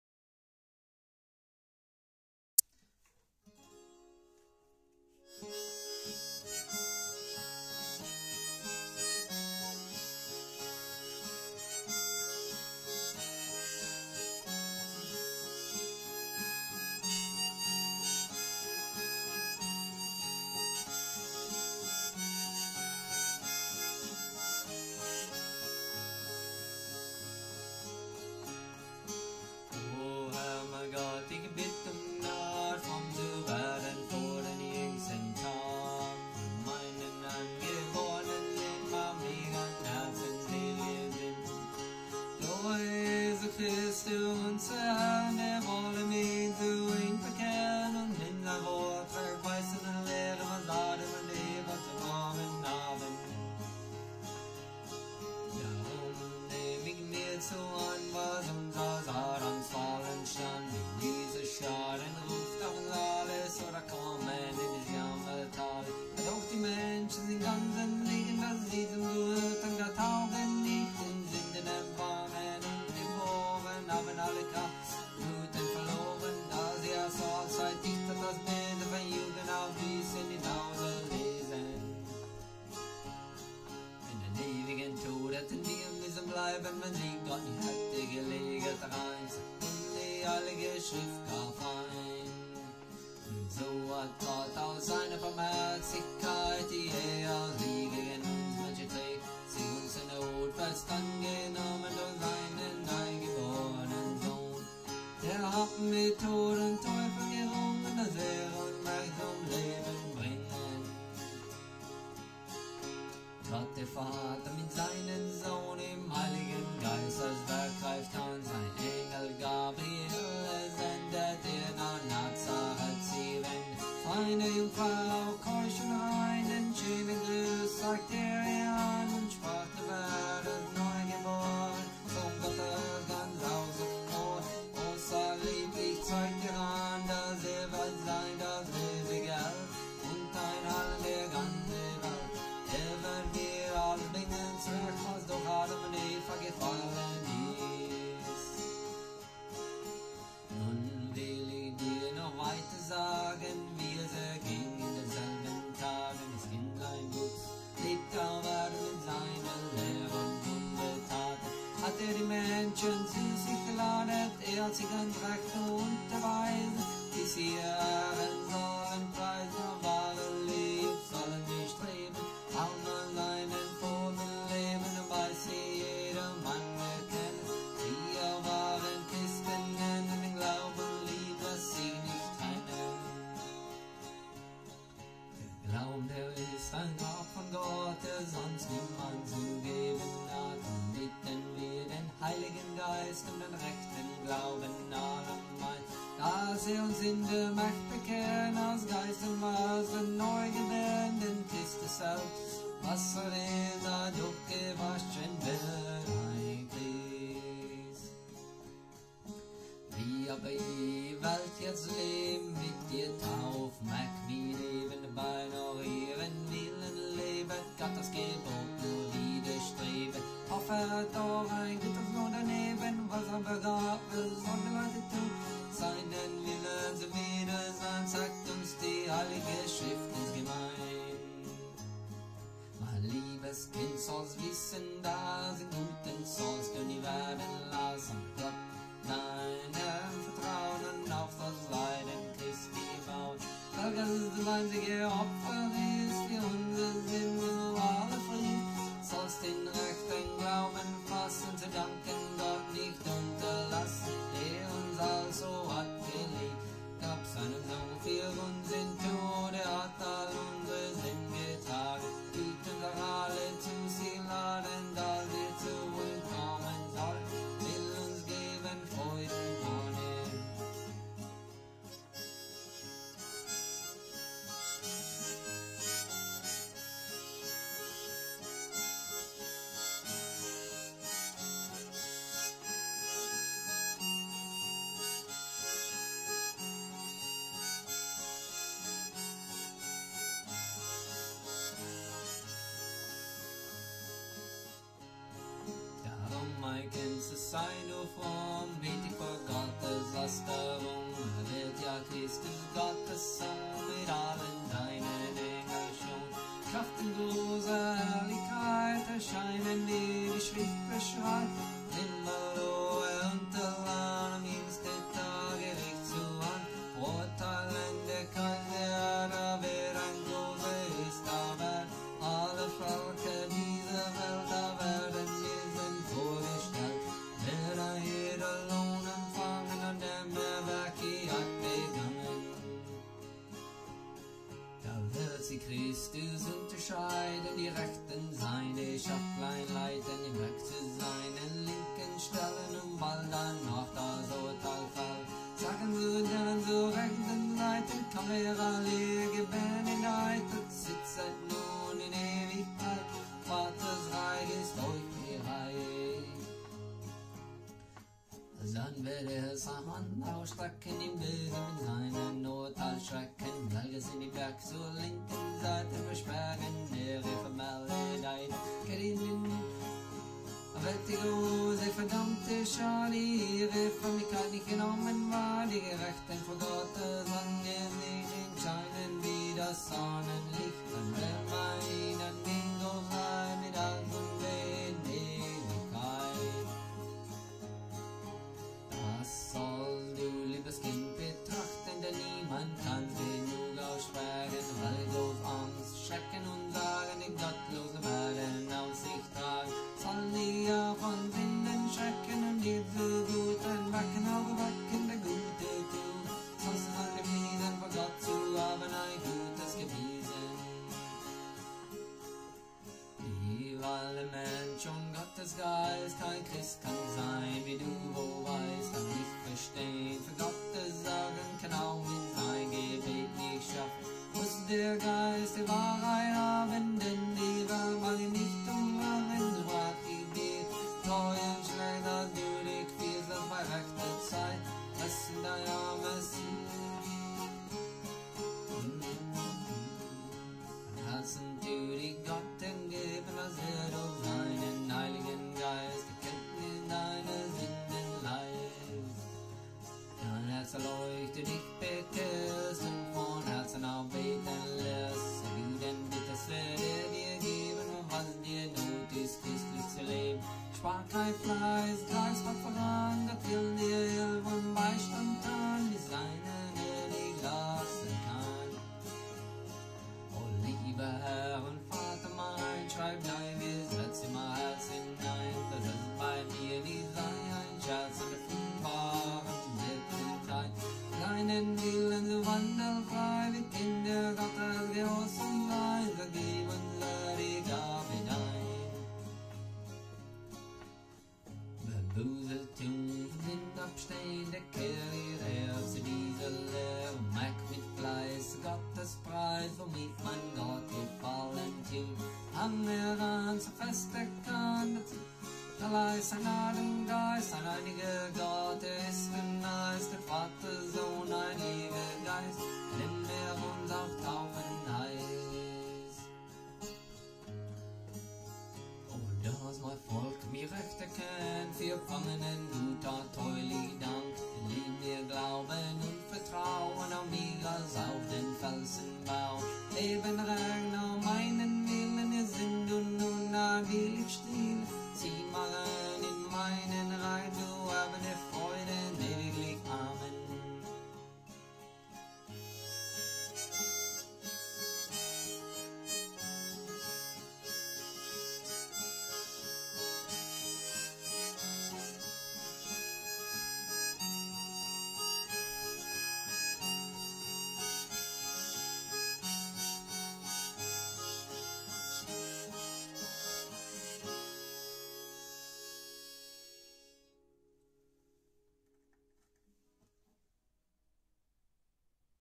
Hutterite Baptism Song
Hutterite baptism poem, put into song.